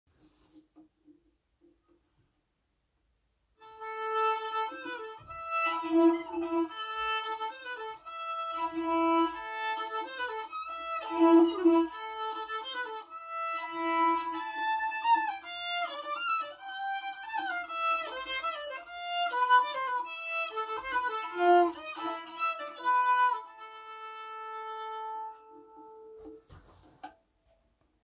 Bowing a Balalaika
I was curious to hear what a balalaika would sound like when bowed, so I experimented a little. The metal A string doesn't sound too bad but the nylon E string is fairly scratchy and difficult to get a nice sound out of. Enjoy!